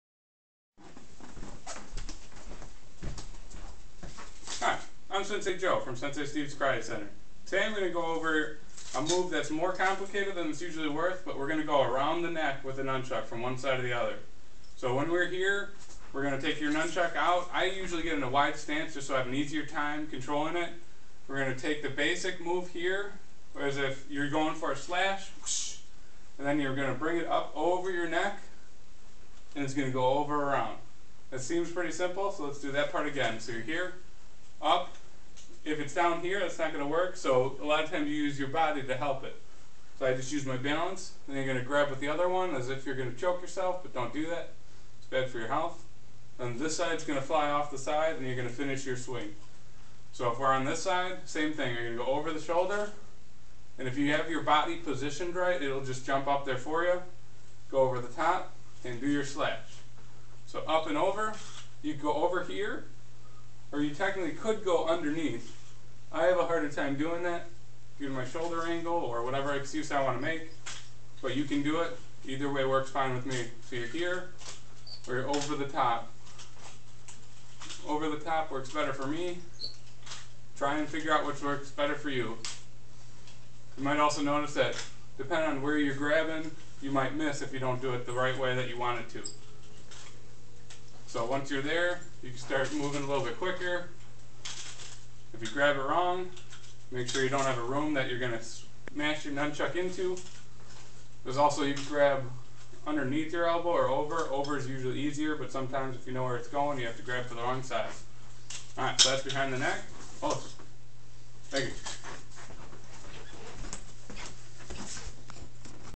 Learn Nunchuck hand change behind neck
This trick is something that may seem hard at first but when you break it down into its small parts it is much easier.